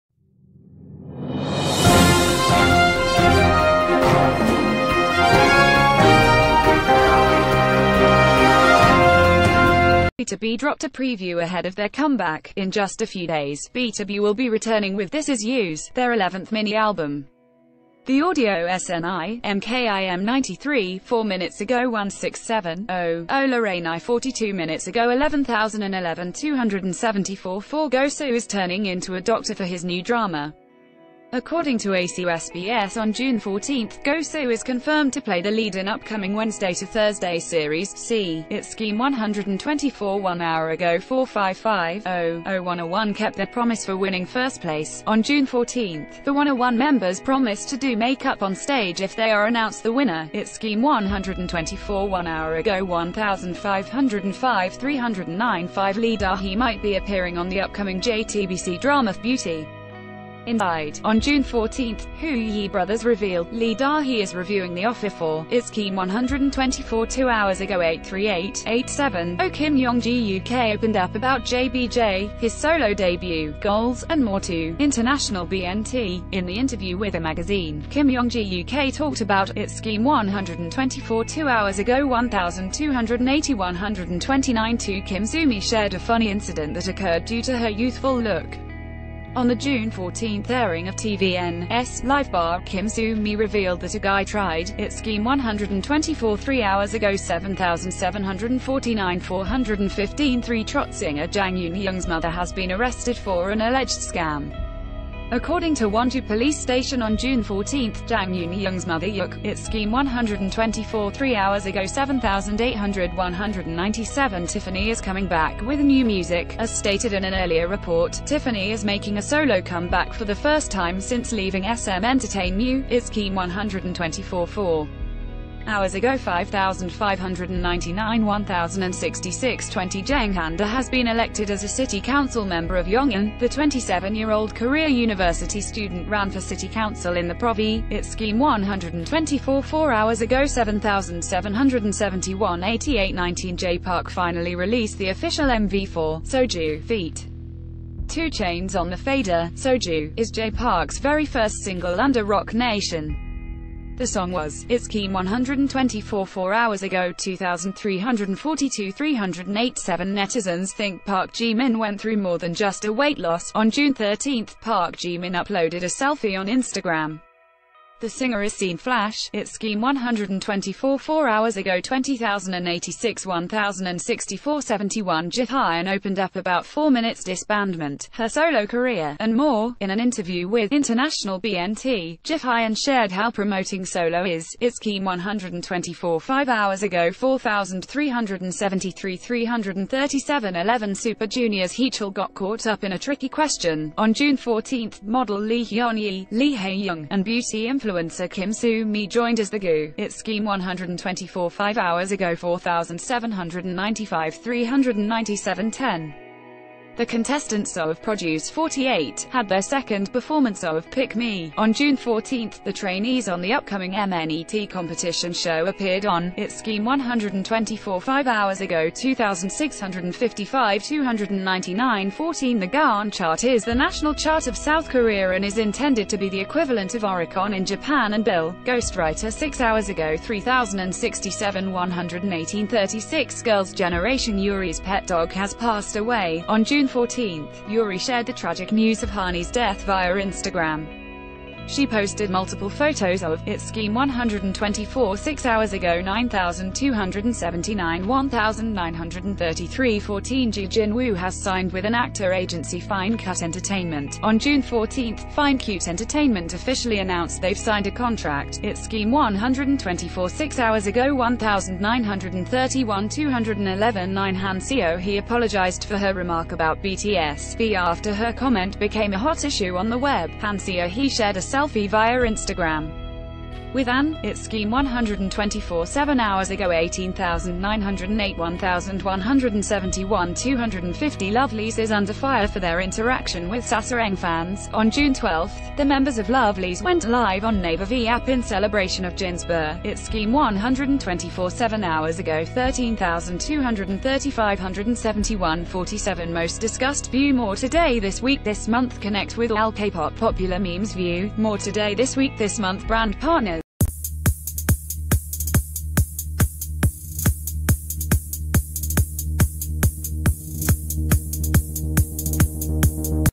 Breaking K-pop news, videos, photos and celebrity gossip
BTOB dropped a preview ahead of their comeback!In just a few days, BTOB will be returning with 'This...